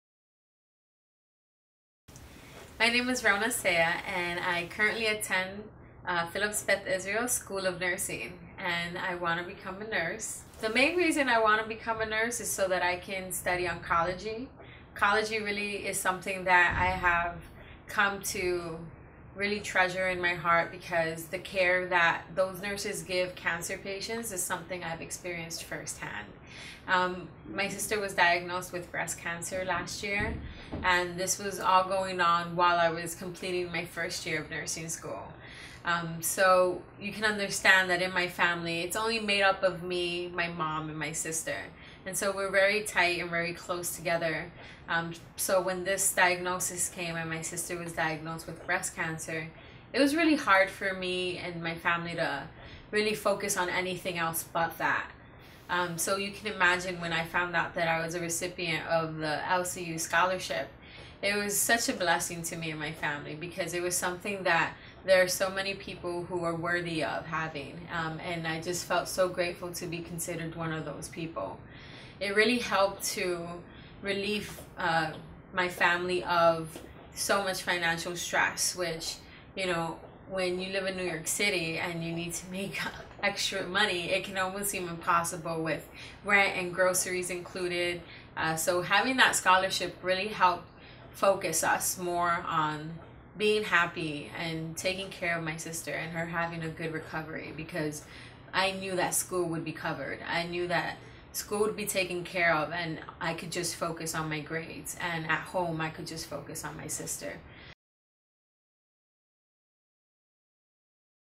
Ronna PBISN
Former LCU Funded student Ronna Cea discusses her preparation to become an oncology nurse.